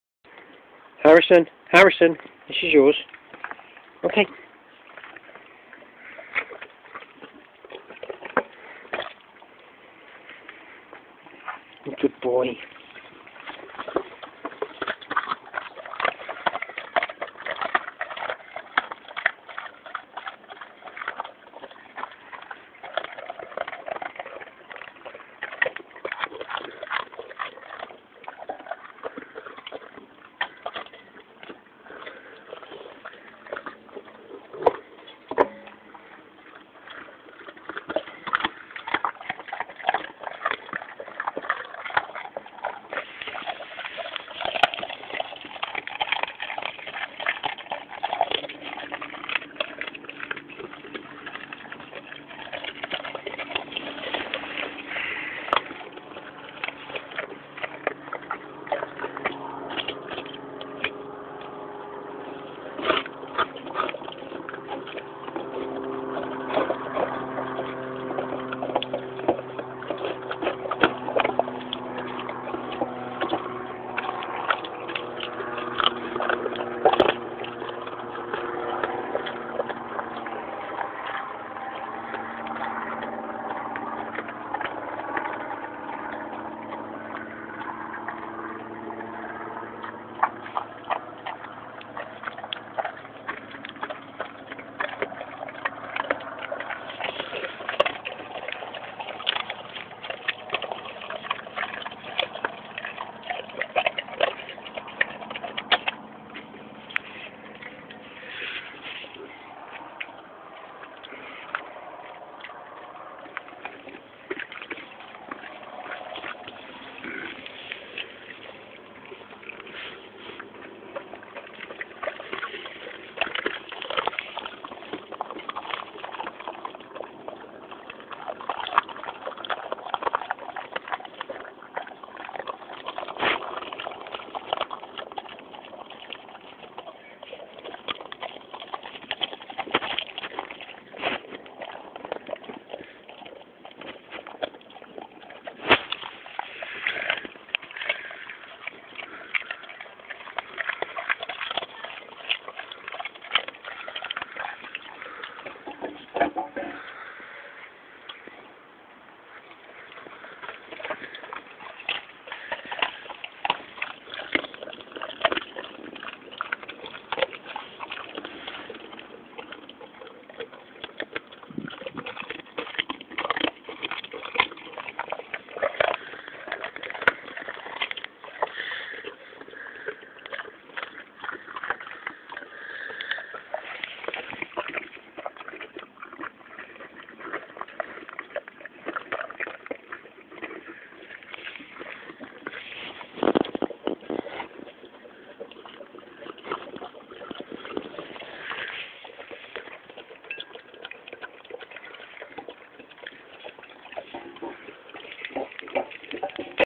English Mastiiffs eating chicken carccasses
My two English Mastiffs, mother and son, eating chicken carcasses